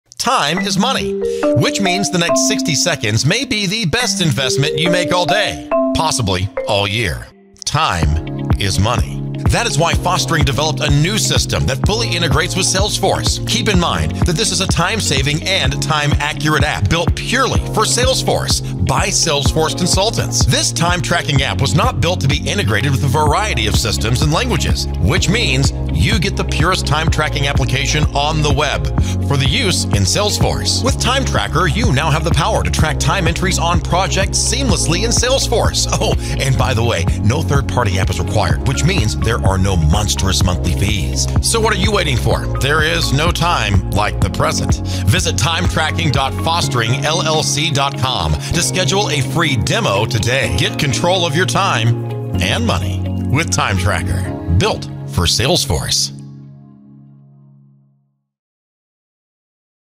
Time Tracking Application for Salesforce | Fostering LLC
Simple Time Tracking Tool Built for Salesforce, by Salesforce consultants and developers. Tired of using multiple systems to track time? 100% Native Salesforce Solution. Your Salesforce can now track time!